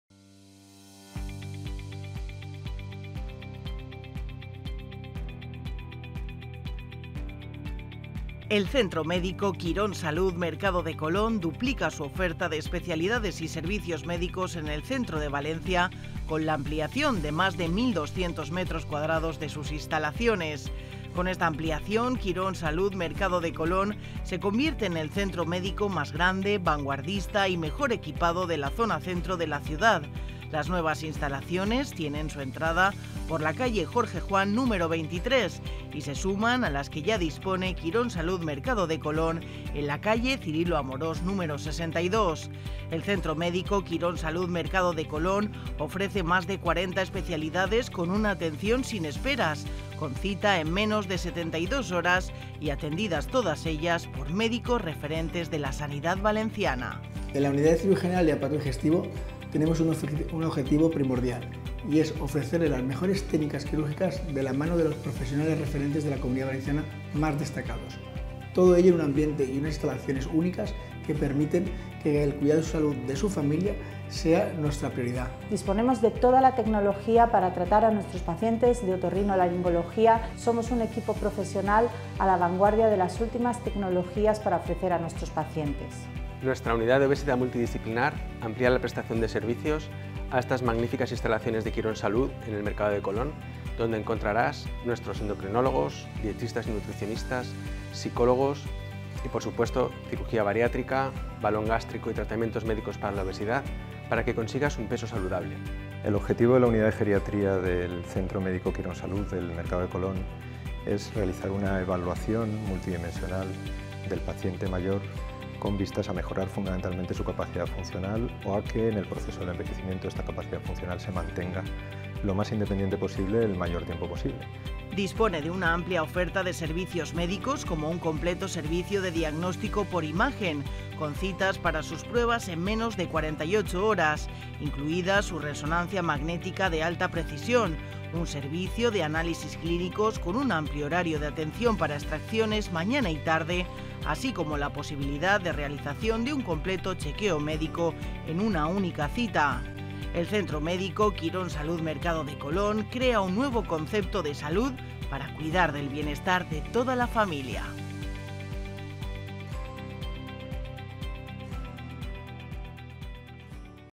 El centro médico Quirónsalud Mercado de Colón duplica sus especialidades y servicios médicos
El Centro Médico Quirónsalud Mercado de Colón ofrecerá más de cuarenta especialidades, todas ellas atendidas por médicos referentes de la sanidad valenciana. En lo relativo a su equipamiento tecnológico el Centro Médico Quirónsalud Mercado de Colón suma a su completo servicio de diagnóstico por la imagen una resonancia magnética de alta precisión. Además, su servicio de Ginecología y Obstetricia incorpora también un ecógrafo de diagnóstico prenatal 4D que permite realizar imágenes del feto a tiempo real en tres dimensiones y ayudar a detectar posibles complicaciones médicas.

El Centro Médico Quirónsalud Mercado de Colón dispone de un servicio de Análisis Clínicos con un amplio horario de atención y punto de extracción continuado, así como una Consulta de Enfermería para curas y la administración de vacunas e inyectables.